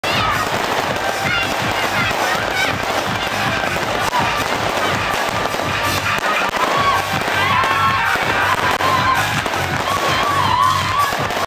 2024/02/25 20:45:36 台東元宵遶境，鋼管女子舞蹈表演
台東元宵遶境第二天晚上在新生路與正氣北路交叉口附近的公益彩券行，豐里代巡府鋼管女子舞蹈表演。